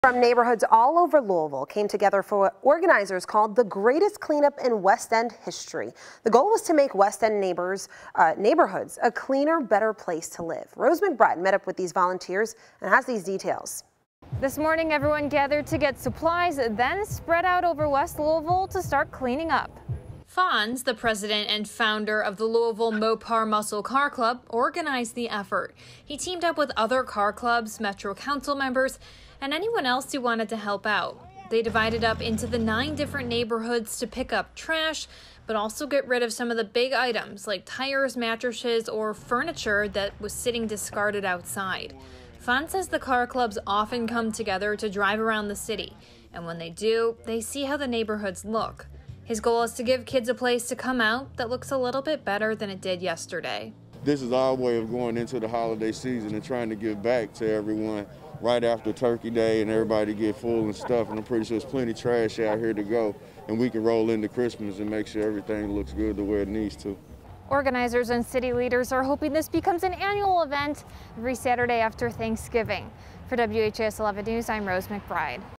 Volunteers make the 'Greatest Cleanup in West End History' happen
The goal was to make West End neighborhoods a cleaner, better place to live. It was organized by car clubs.